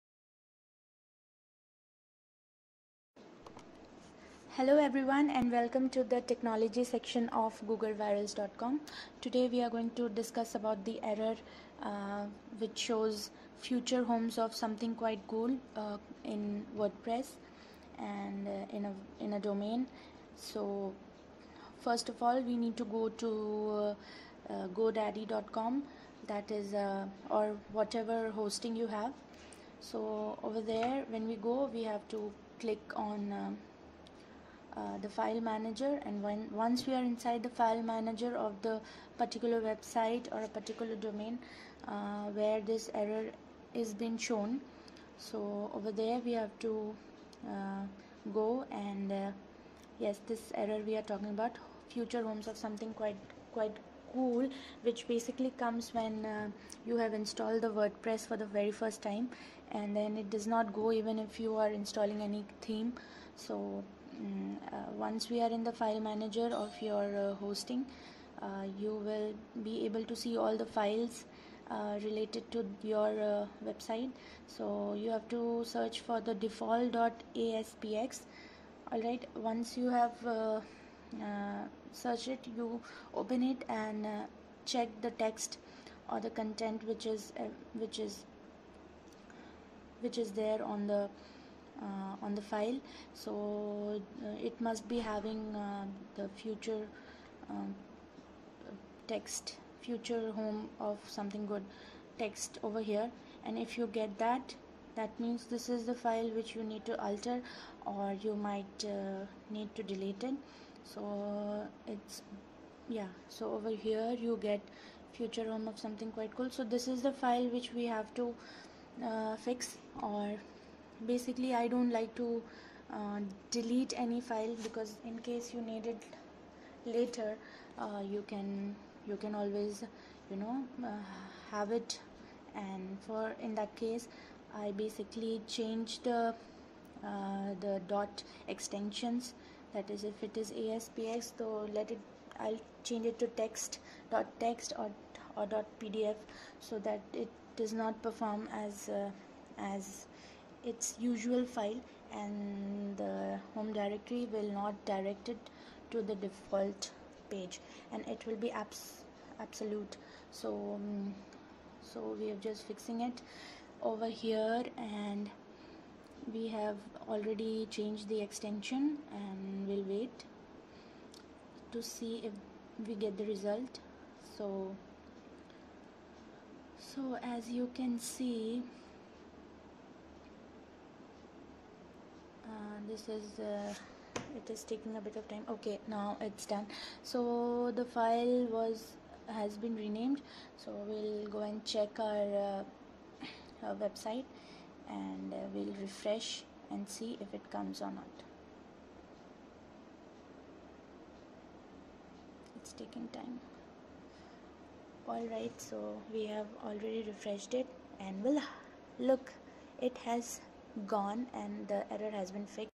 Debug "Future home of something quite cool" in domain| Wordpress| Godaddy
Hi there, After settled up my sit,  i was facing this error “Future home of something quite cool. Wordpress| Godaddy
Initially, I was not able to detect the issue of why this error started appearing on my homepage. Later when I check all details on my hosting under the File Manager I found the error and then the problem was solved with simple steps. So guys, Check out the below steps to solve the ‘Future Of Something Quite Cool’ error.